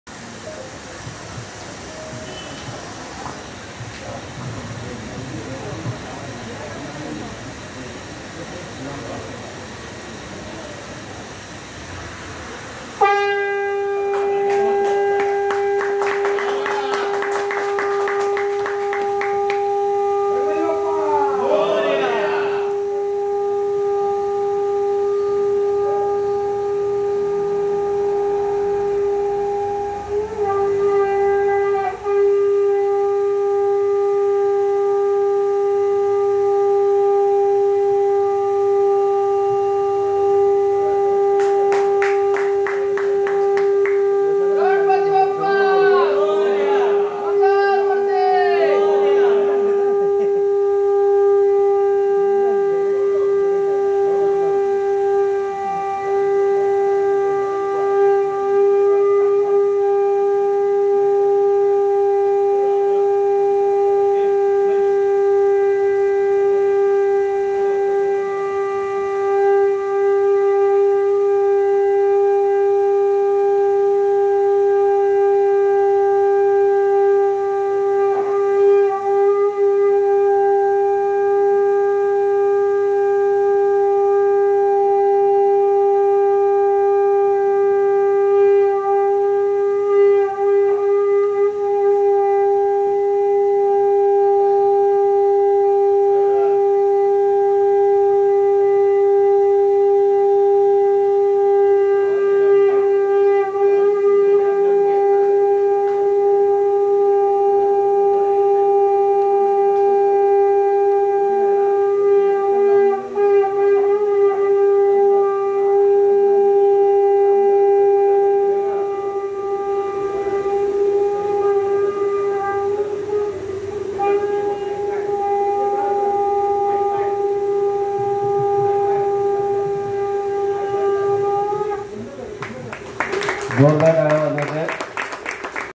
महा शंखनाद Maha Shankhnaad || गोकुलधामचा राजा मुंबई Gokuldhamcha Raja Mumbai
2 minute tak Shankhnaad
दो मिनिट तक शंखनाद
गोकुलधामचा राजा मुंबई
Gokuldhamcha Raja Mumbai